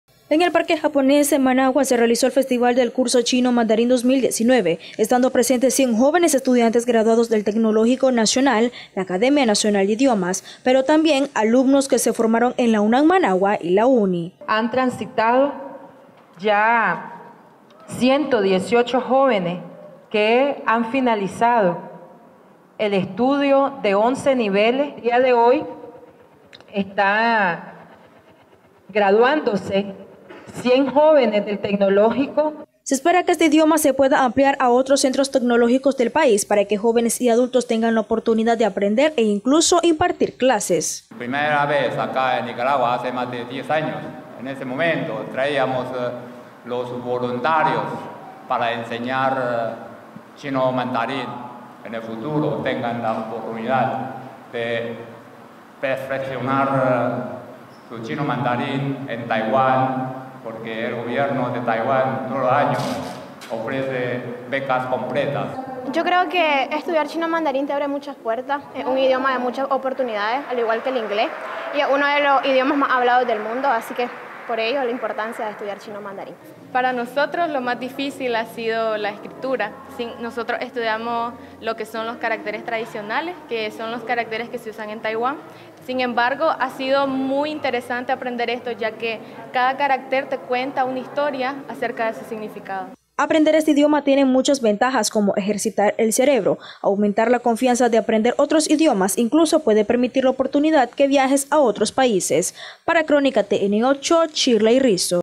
100 jóvenes se gradúan del festival del curso Chino-Mandarín - Nicaragua
En el Parque Japonés en Managua, se realizó el festival del curso chino-mandarín 2019, estando presentes 100 jóvenes estudiantes graduados del Tecnológico Nacional, la Academia Nacional de Idiomas, pero también alumnos que se formaron en la UNAN-Managua y la UNI.